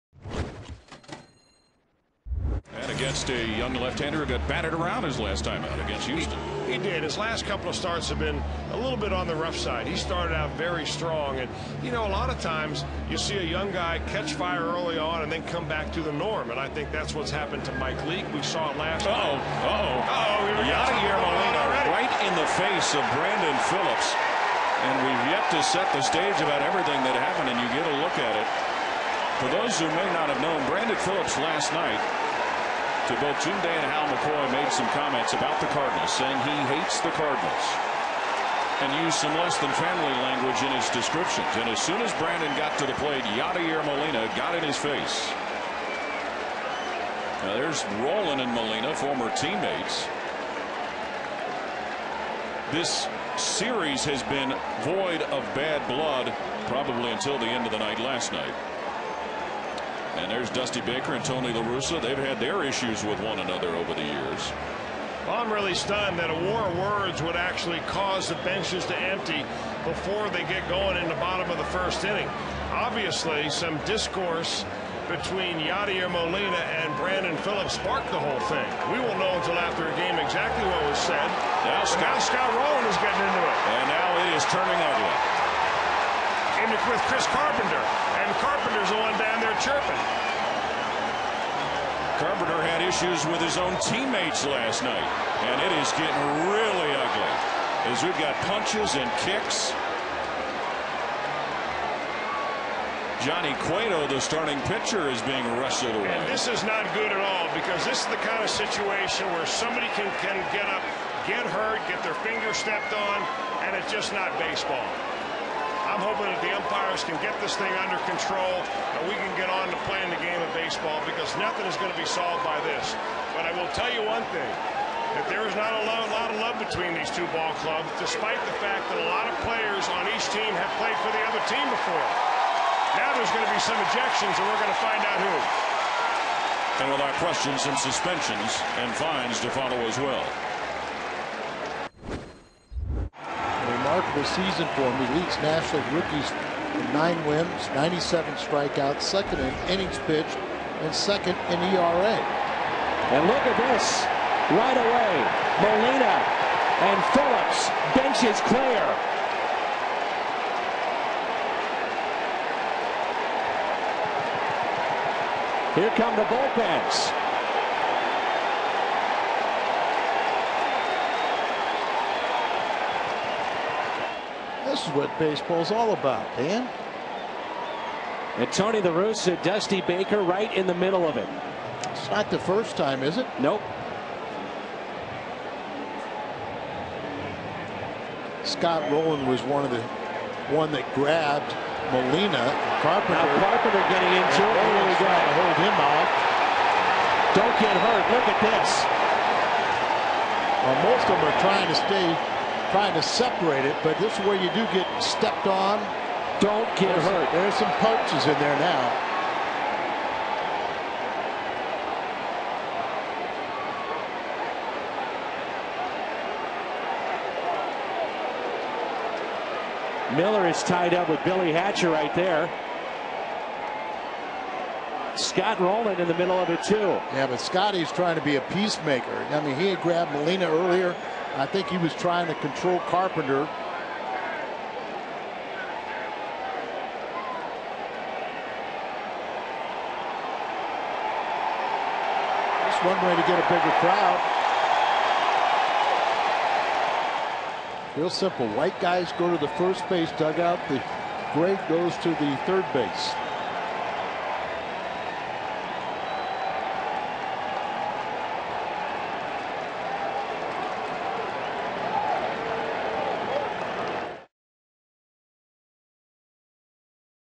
Cardinals vs Reds The Fight 08/10/10
This fight started because some comments made by second baseman Brandon Phillips ... this is how this started!